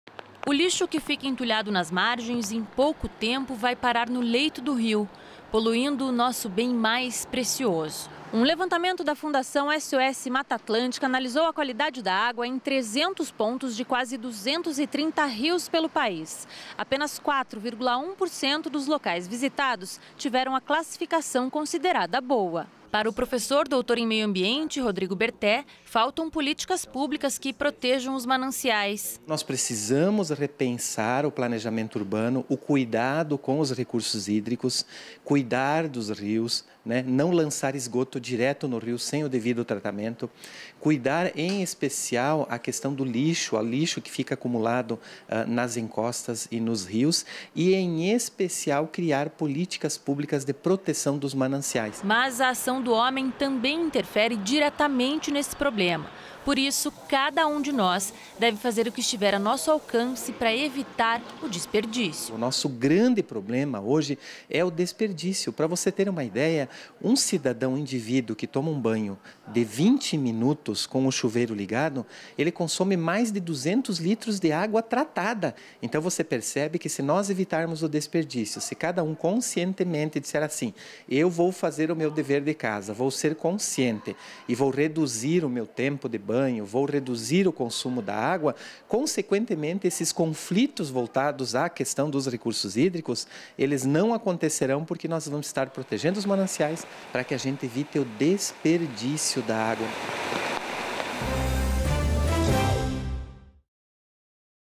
JORNALISMO | POLUIÇÃO DA ÁGUA PREOCUPA [CC]
UM LEVANTAMENTO DA FUNDAÇÃO SOS MATA ATLÂNTICA CONCLUIU QUE EM 20% DOS PONTOS ANALISADOS, A ÁGUA É RUIM OU PÉSSIMA. SÓ EM 4% DOS LOCAIS A QUALIDADE É BOA.

JOR18VT0220DIAMUNDIALDAAGUA